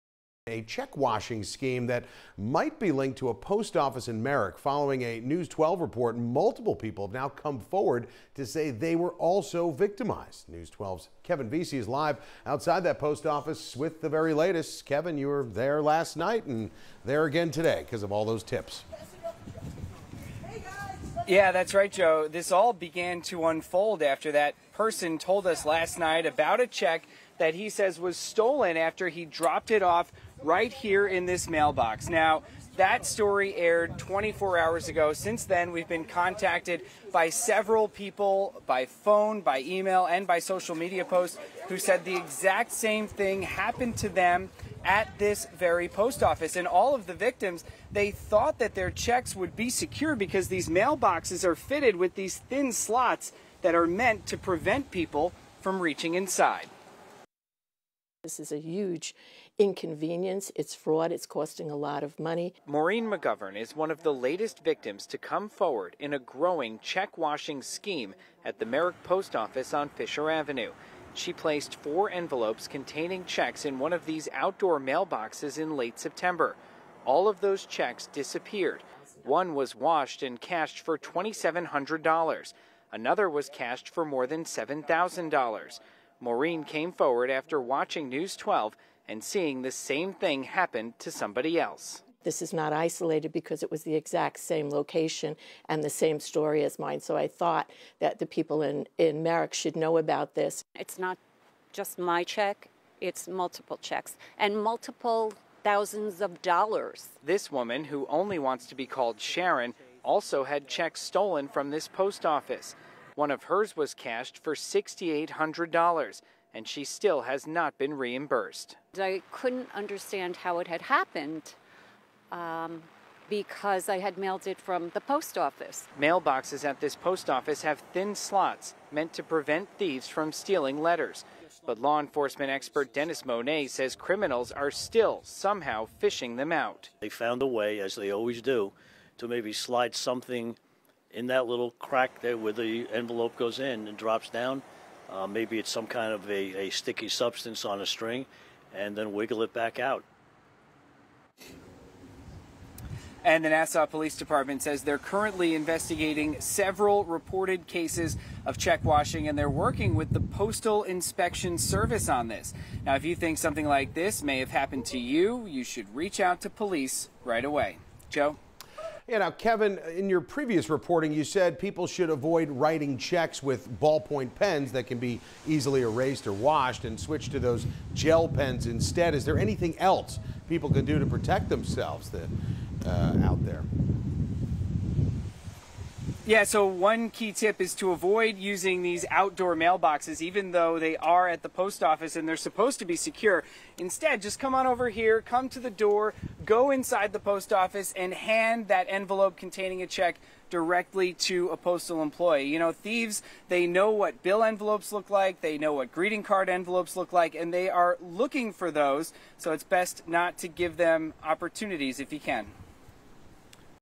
Victims come forward in alleged check washing scheme at Merrick Post Office
News 12 first reported about the issue Thursday after a man said his check was stolen after he dropped it into the mailbox on Fisher Avenue.